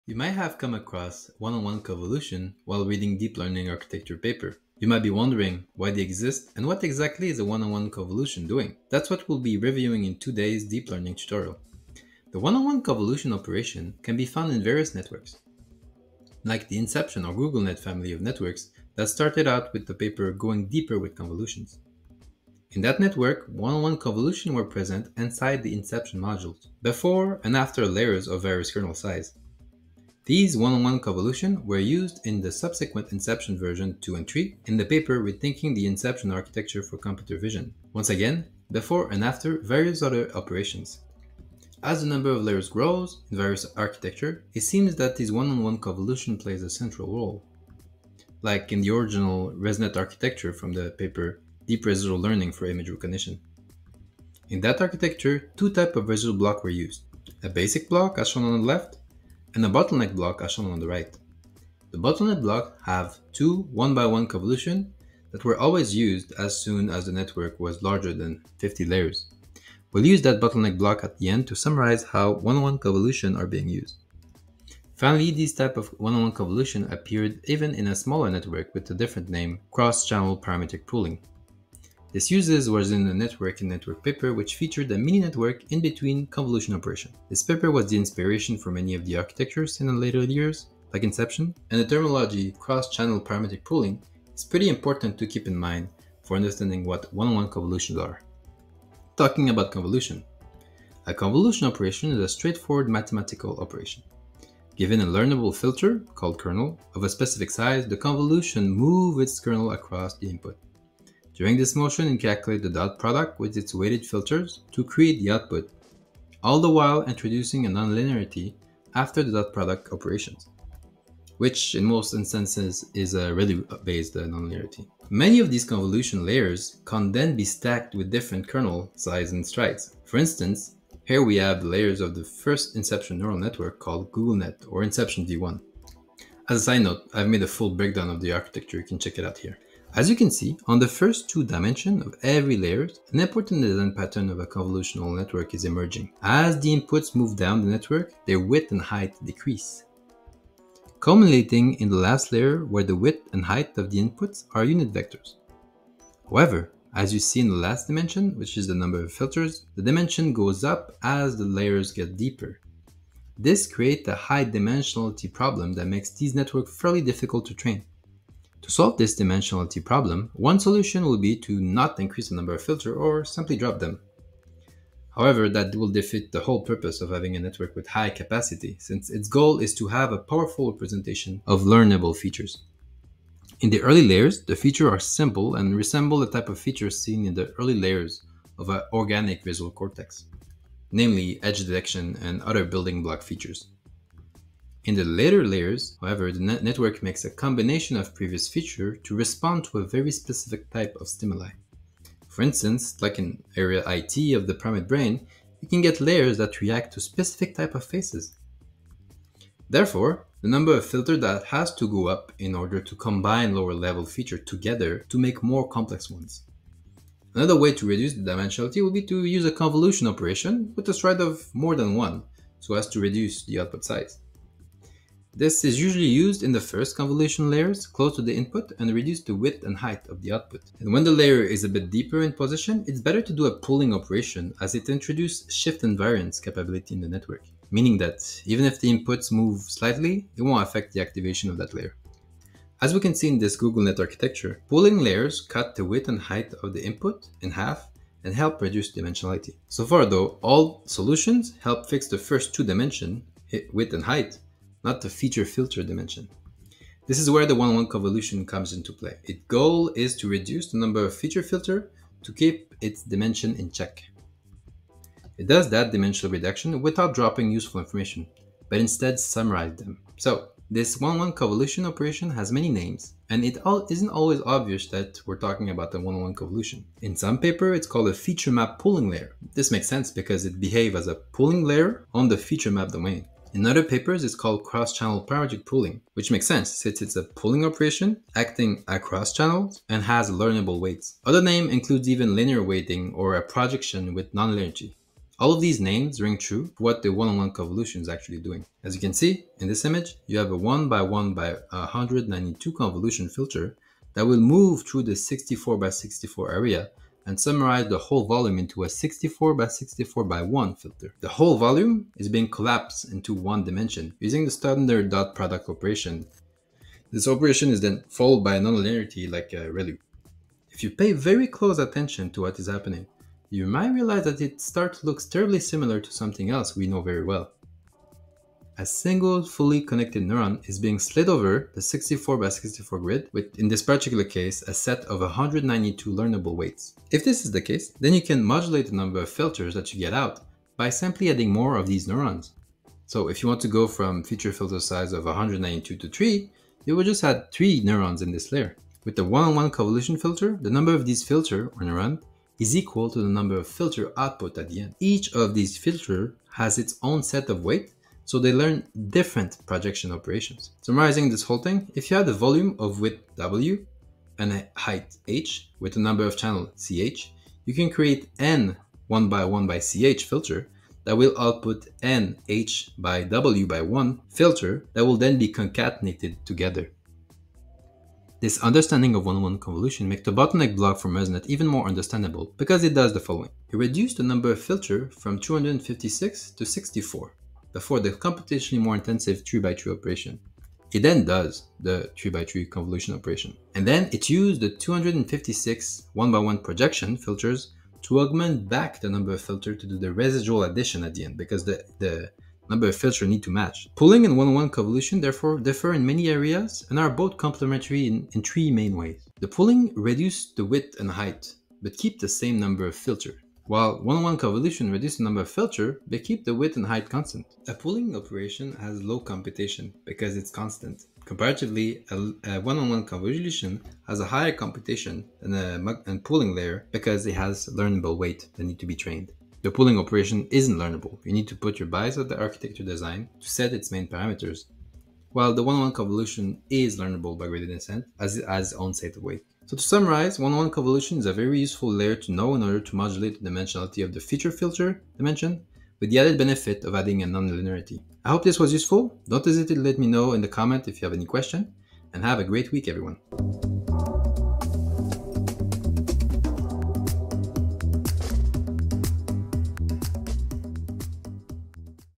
What are 1x1 Convolutions in Deep Learning?
👨‍💻 to get started with AI engineering, check out this Scrimba course

You might have come across 1x1 convolution in deep learning architecture and wondered why they were there. In this tutorial, I'll walk you through their usefulness and how they compare to other dimensionality reduction techniques.

- Introduction: 0:00
- 1x1 in networks: 0:11
- Convolutions: 1:40
- How to reduce dimensionality: 2:48
- What is 1x1 convolution doing?: 4:25
- Pooling vs 1x1 convolution: 6:44
- Conclusion: 7:19

Here are some other resources that are very good on this topic:
📌 How are 1x1 convolutions the same as a fully connected layer? (Another StackOverflow Question)

Really great content, check it out! ☝️